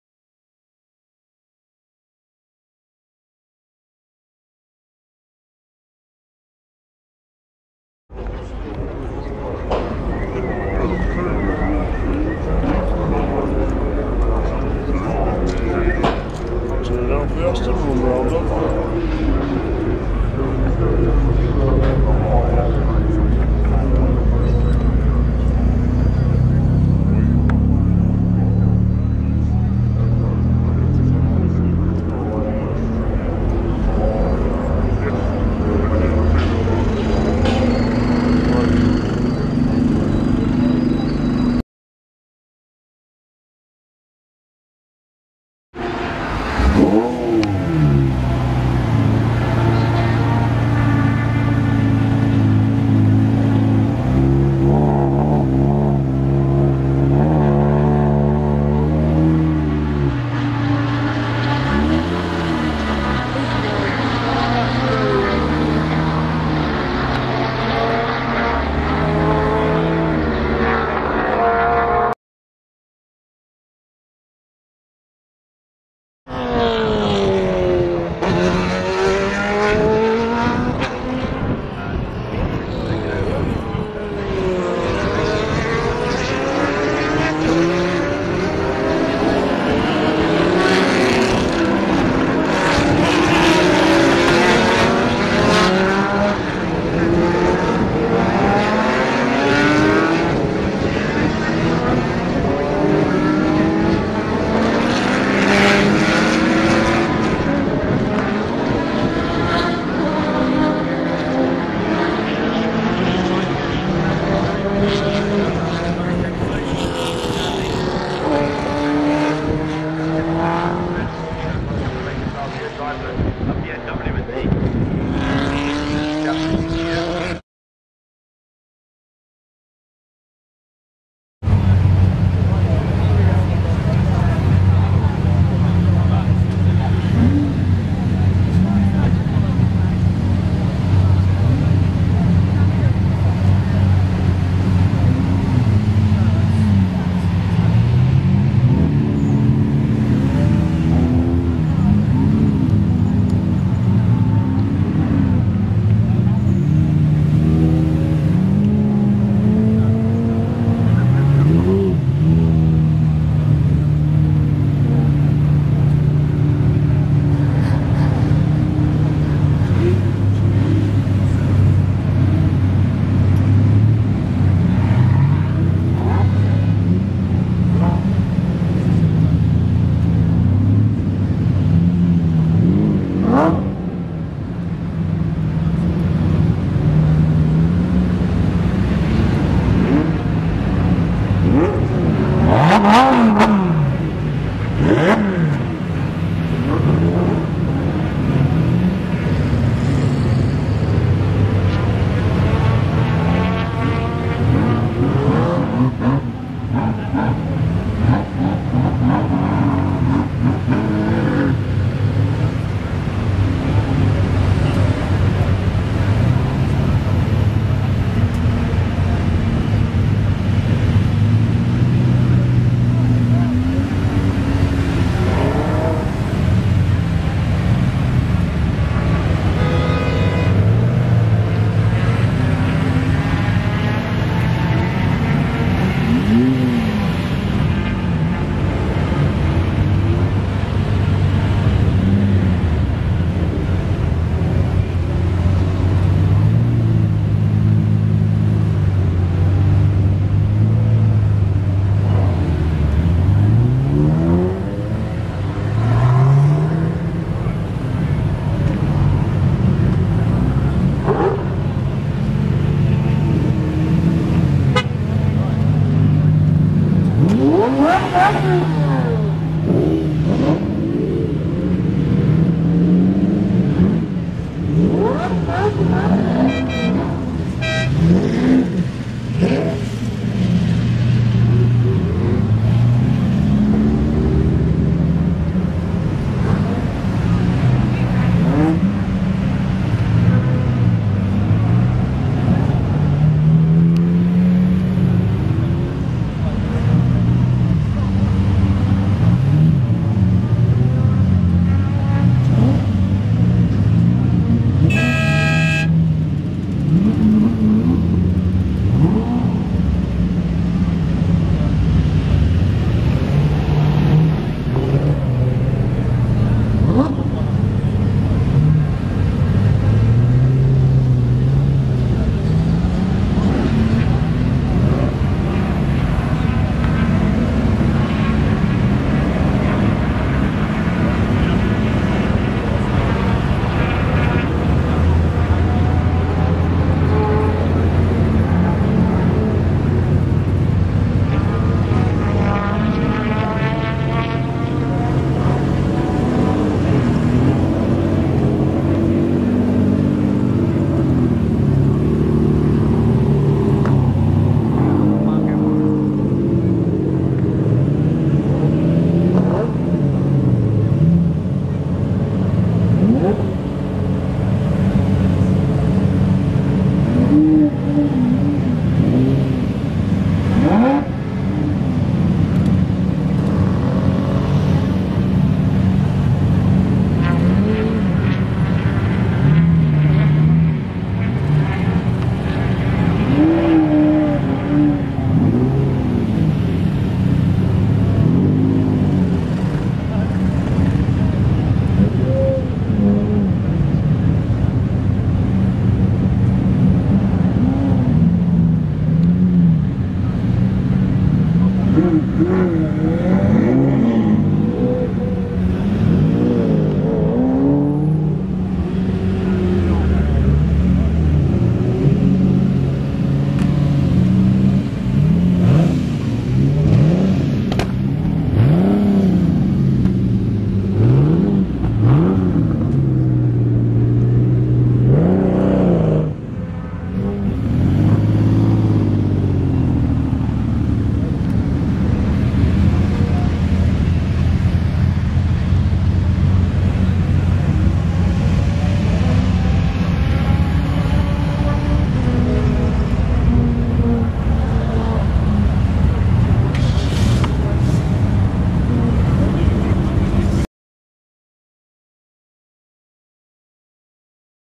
Silverstone2013
Clips from the Silvestone Classic 2013 - Aston Martin, Group C, Lamborgini traffic jam,